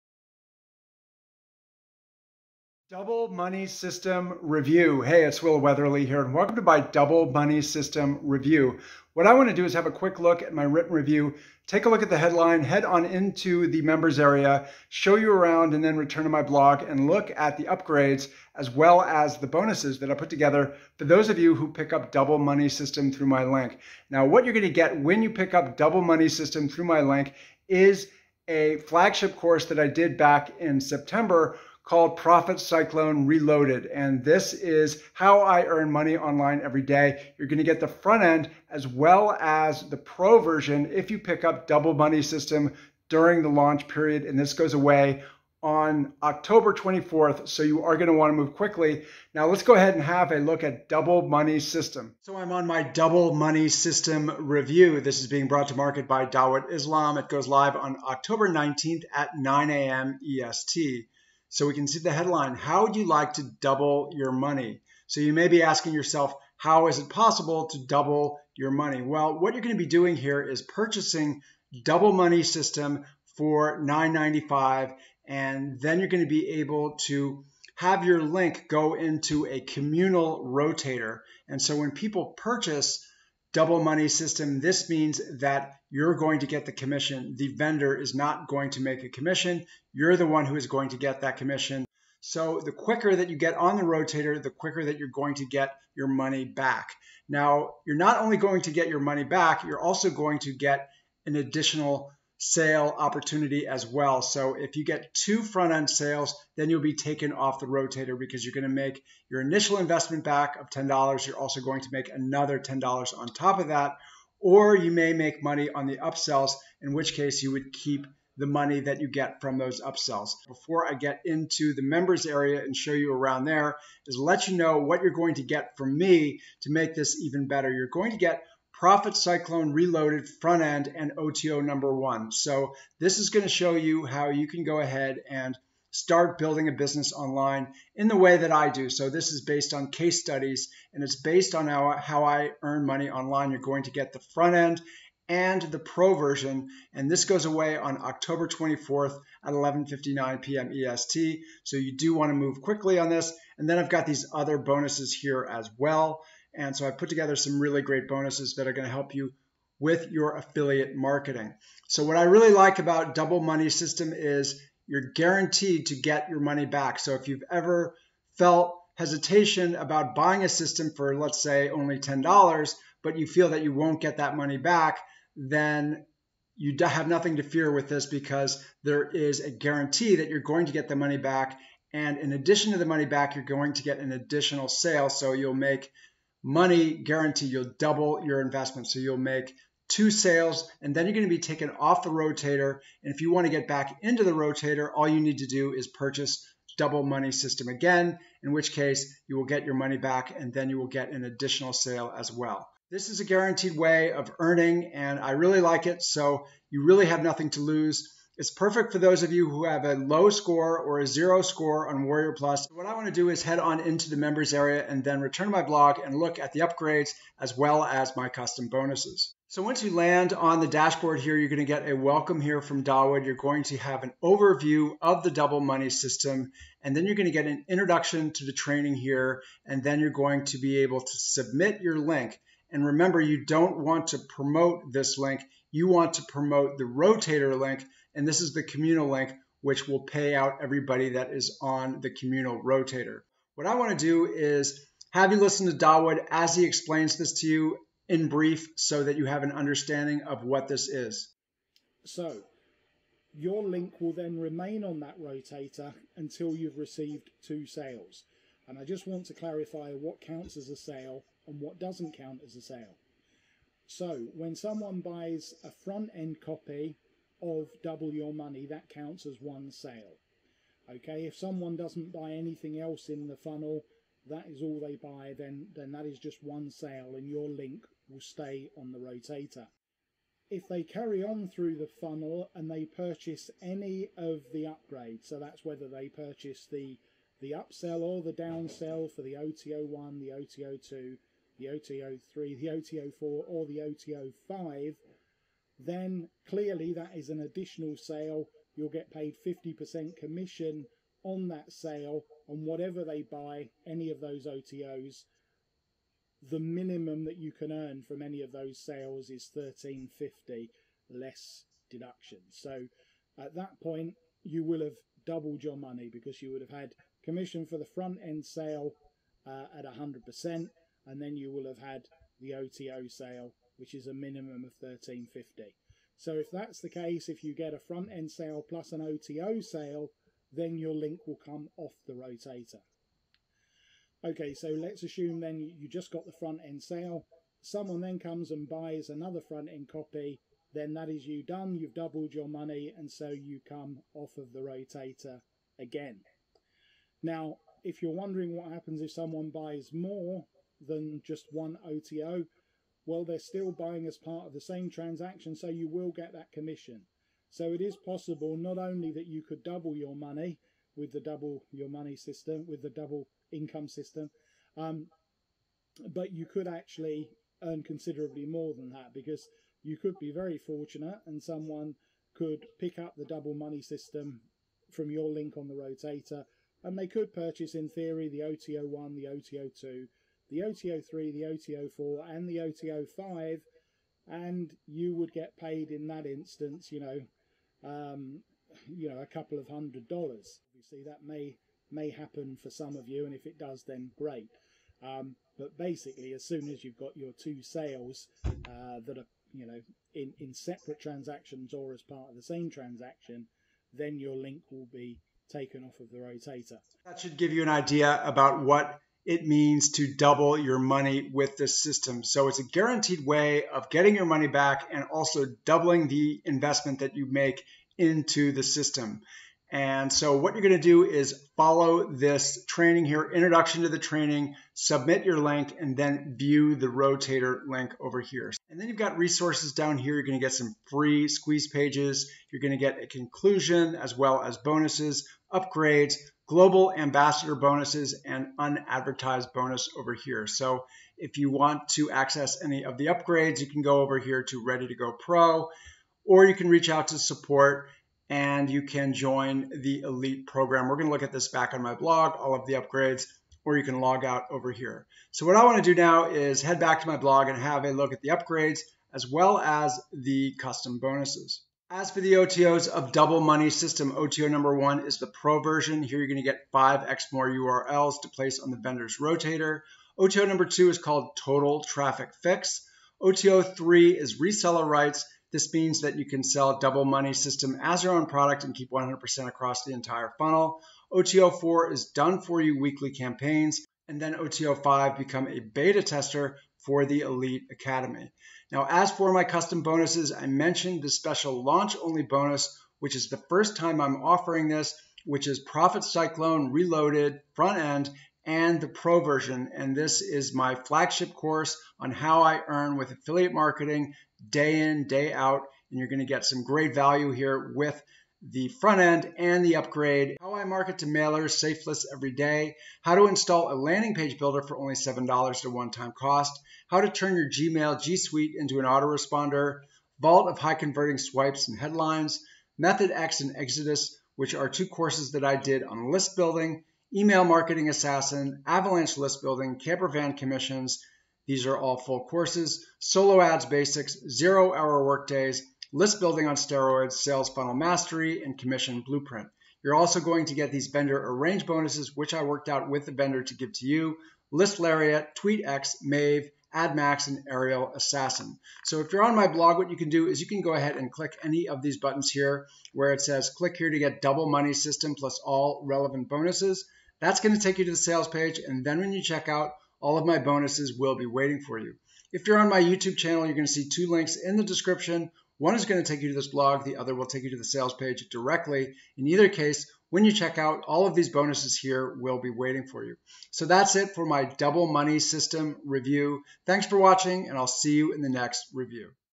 Double Money System Review
Double Money System is a new product by Dawud Islam.
It goes live on October 19th at 9AM EST.
Double Money Review
What Is Double Money System All About?
In a nutshell, with Double Money System you are guaranteed to double your money. That is, you will be able to double your money with passive sales from the vendor's rotator. Your link remains on the rotator until you have had either:
Two front end sales OR one front end sale plus any OTO sale.
(Also, note that in-line add-ons don't count as sales).
If you want to go back on the rotator and repeat this, all you have to do is buy another copy.
What Steps Do You Need To Take?
First, When you enter the members area you will want to watch the welcome and overview videos. Then, go to the main training section consisting of the following:
Introduction to the training
Submit your link
Promote the rotator link
When you submit your link, you will get 100% commissions on the front end.
None of the sales that come in go to the vendor.
Instead, they will go directly to those who bought the system.